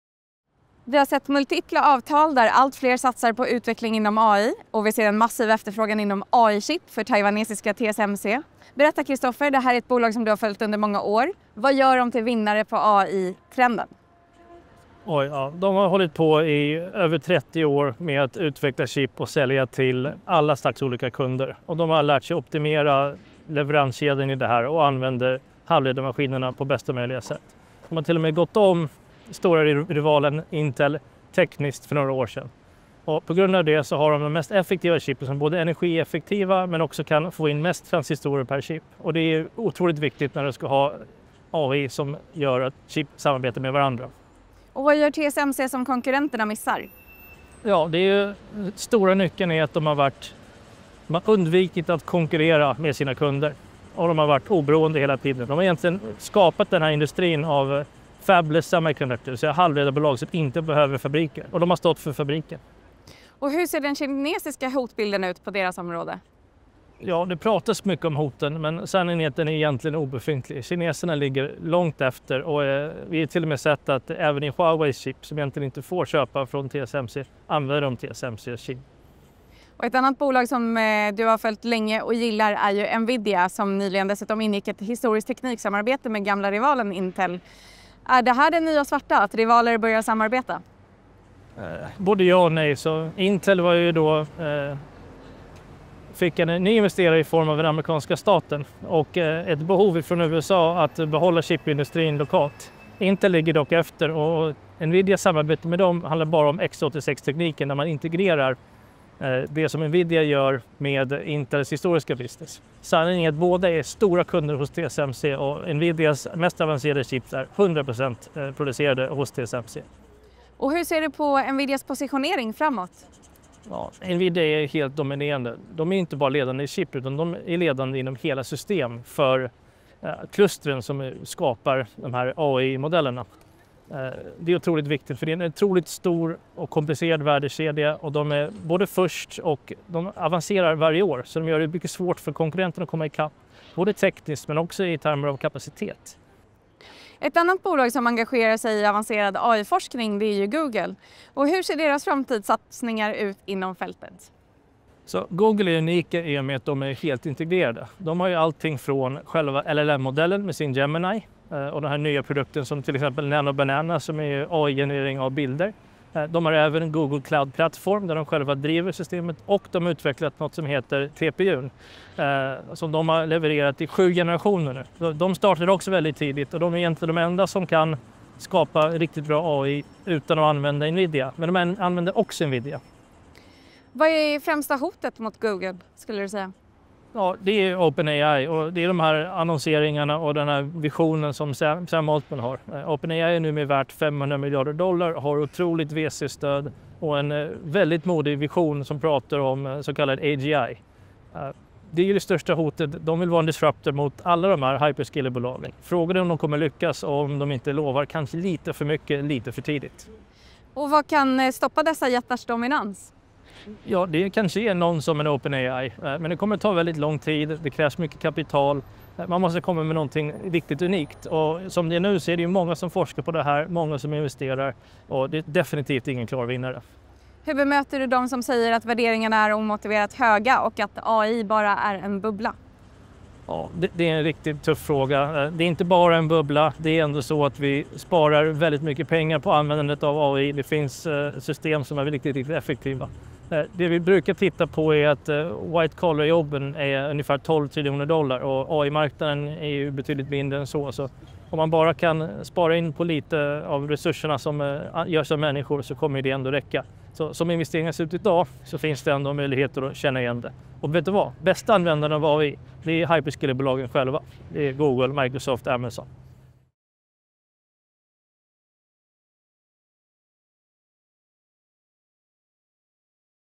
Förvaltaren: "Nvidia är helt dominerande"
En våg av historiska avtal och investeringar håller på att omforma hela tekniksektorn, driven av den snabbt växande globala efterfrågan på AI-teknik.

Satsningar inom AI-forskning skapar tillväxtmöjligheter för bolag som TSMC, Nvidia och Google. Vad är det som gör dessa företag världsledande – och hur lyckas de behålla sin position i den allt hårdare konkurrensen? Kristofer Barrett, förvaltare vid Carmignac, delar sin syn och berättar mer om hotet från Open AI.